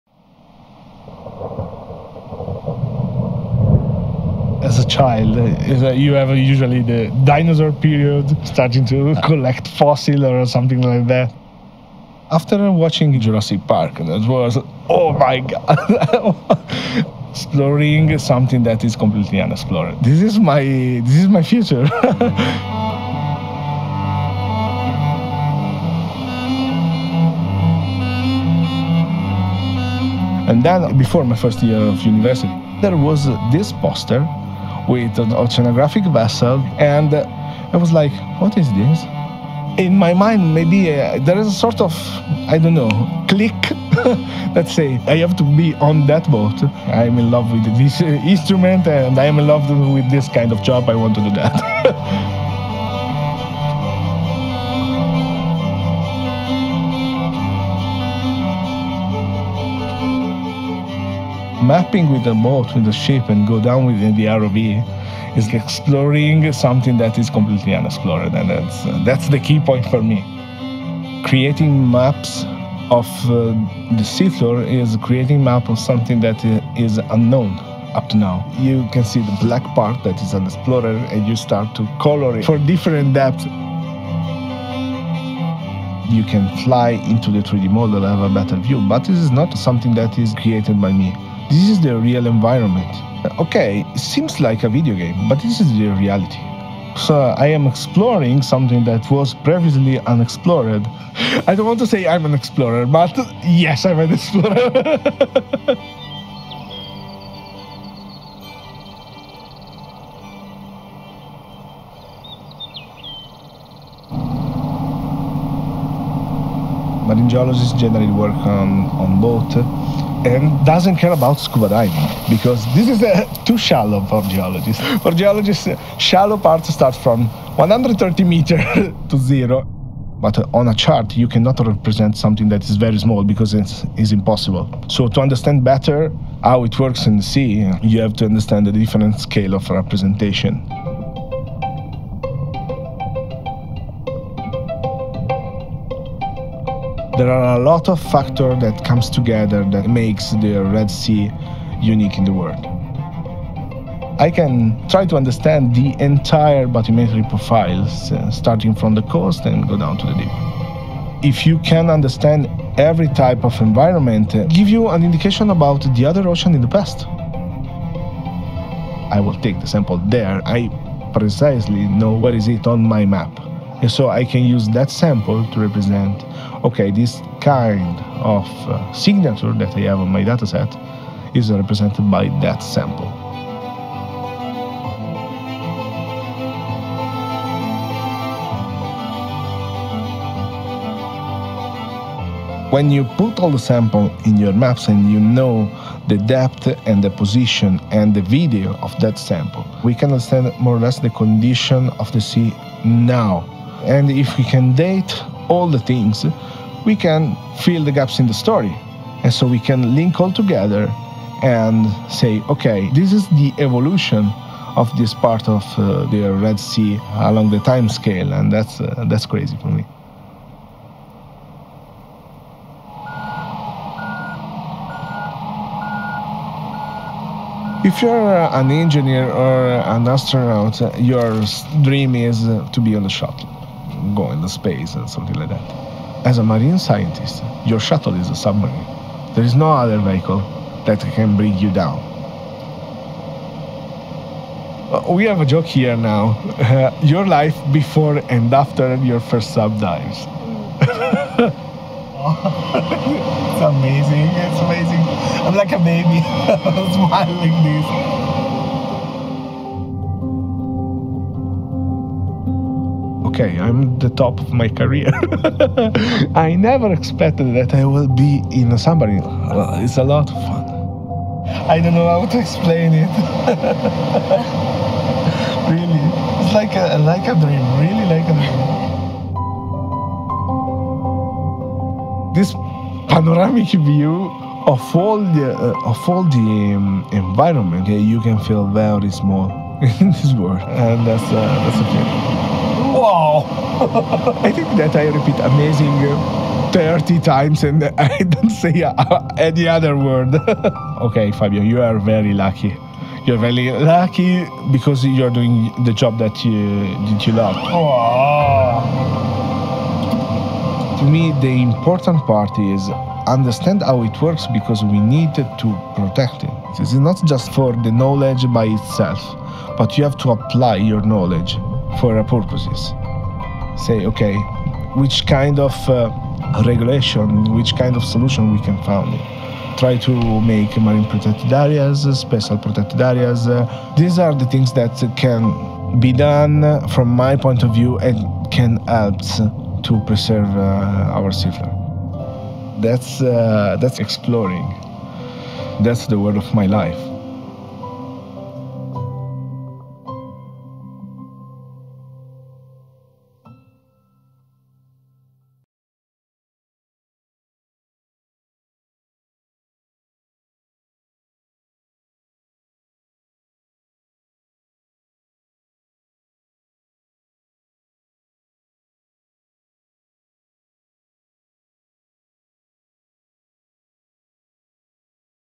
Visiting The Unexplored Seafloor
Compared to other ocean basins, the Red Sea is very young. This makes it an exciting study subject for marine geologists, as it's one of only a few places on Earth where they can observe the early stages of the formation of an ocean.

Aboard the OceanXplorer, marine geomorphologist Fabio Marchese is on an unprecedented mission to explore the depths of the Red Sea. To achieve this, he combines high-tech tools, such as an acoustics dataset provided by echosounder, sonar, and a sub-bottom profiler, with direct observation of the environment through diving and sampling.

His work will give both geologists and biologists a fuller picture of an uncharted part of the earth, the deep Red Sea.

Director: Janssen Powers
Executive Producers: Mark Dalio, Joe Ruffolo, Jonathan Schienberg
Senior Producer: Erika Jarvis
Producers: Mika Chance, Carly Tarricone
Story Producer: Lindsay Blatt
Series DP: Ivan Agerton
Underwater Series DP: Sean Ruggeri
DP: Janssen Powers
Post Production Supervisor: Gabe Tanenhaus
Editor: Janssen Powers
Assistant Editors: Patrick Hopkins, Carly Tarricone
Sound: Janssen Powers
Color Grade: Running Man Post
Sound Mix: Max Sound Recording